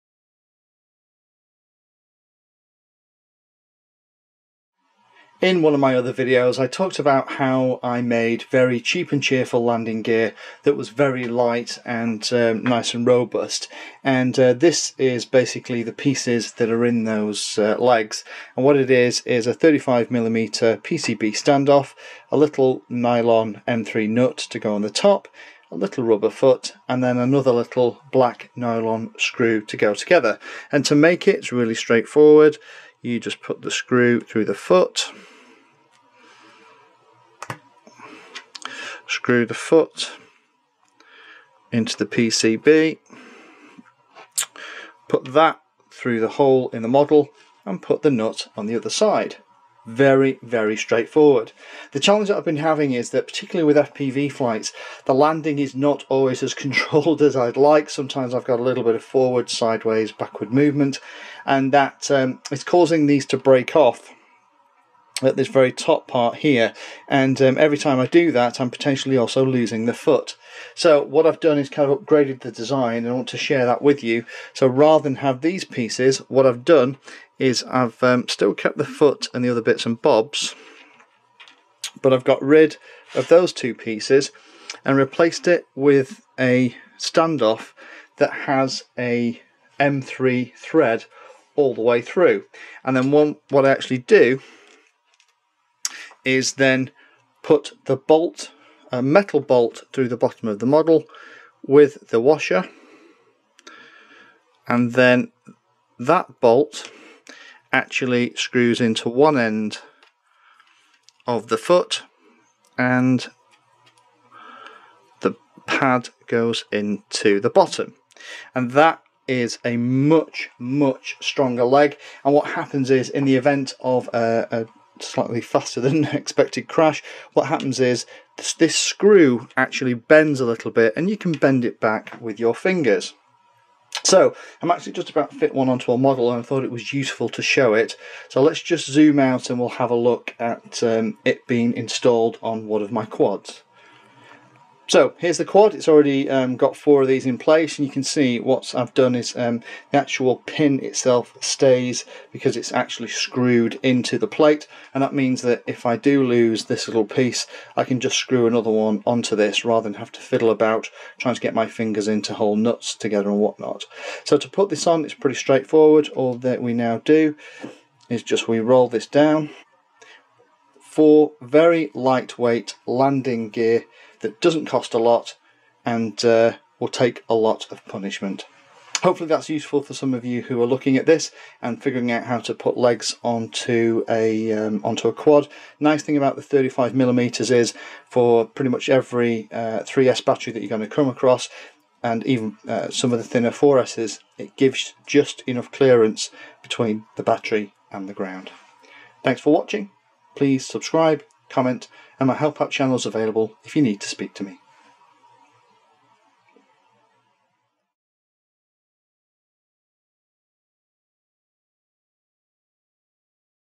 Cheap, stong, light landing gear for quadcopters and multirotors
In another video I shared how I made very light, cheap landing gear using PCB standoffs and small rubber feet. I've had a few snap where the PCB standoff connects through the model and I now use this design where a small M3 screw holds the leg in place.

In the event of a crash the screw bands and can be bent back into shape easily using your hands. This version is much stronger and means that you don't loose the rubber feet every time the gear takes a rough landing.

Happy flying.